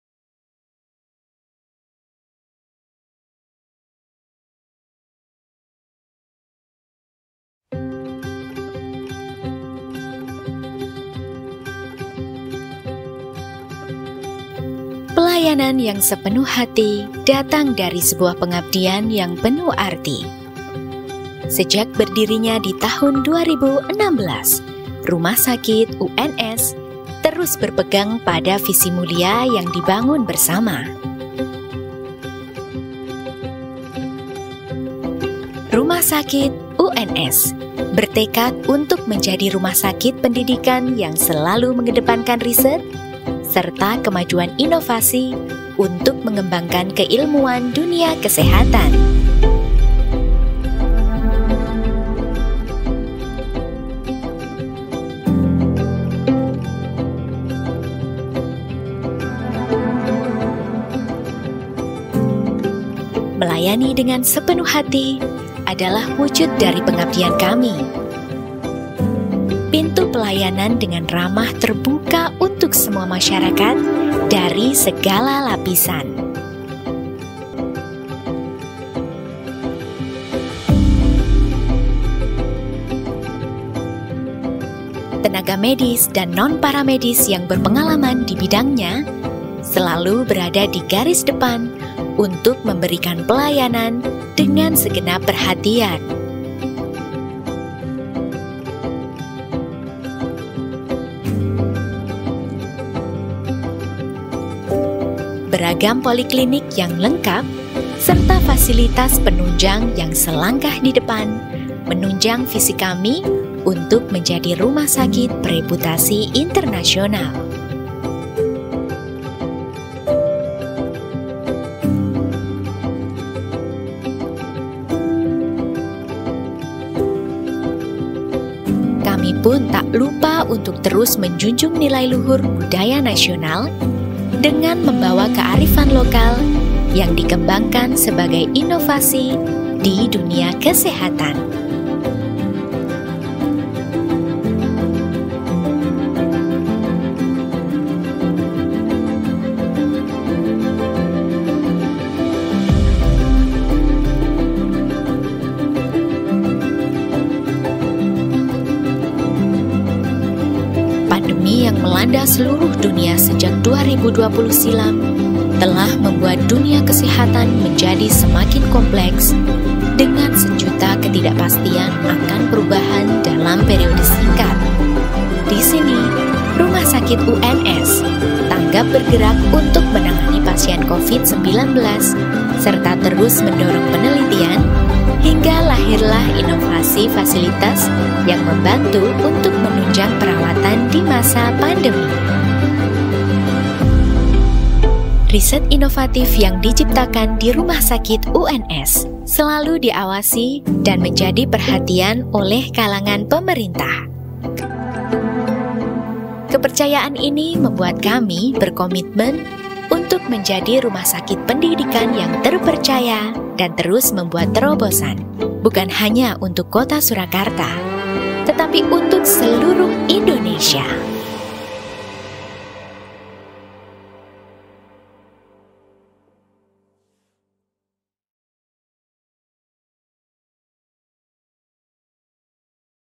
PROFIL RS UNS 2022
Rumah Sakit UNS adalah rumah sakit yang mempunyai fungsi sebagai tempat pendidikan, penelitian, dan pelayanan kesehatan secara terpadu dalam bidang pendidikan kedokteran dan / atau kedokteran gigi, pendidikan berkelanjutan, dan pendidikan kesehatan lainnya secara multiprofesi. Dengan memiliki Visi

“Mewujudkan RS UNS sebagai pusat pendidikan, penelitian dan pelayanan kesehatan, bereputasi internasional, berlandaskan kedokteran komunitas dan nilai luhur budaya nasional”